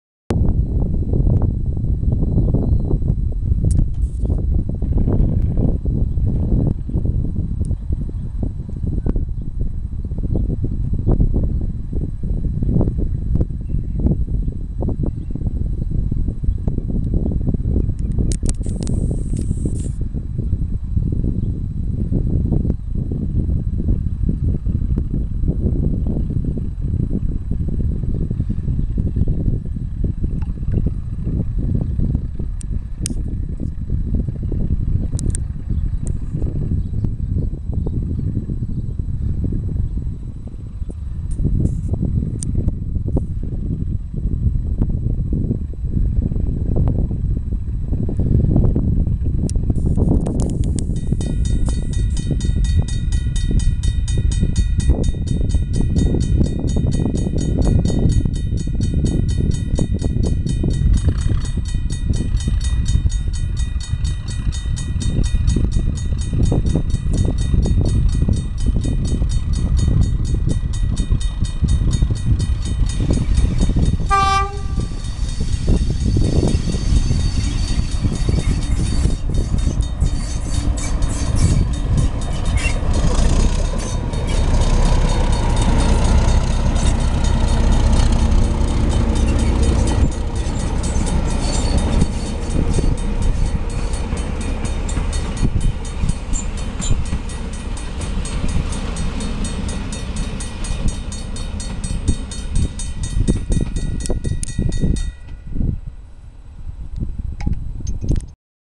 DE 511 at Ferrymead 9/8/20.
DE 511 at Ferrymead Heritage park Sunday 9/8/20.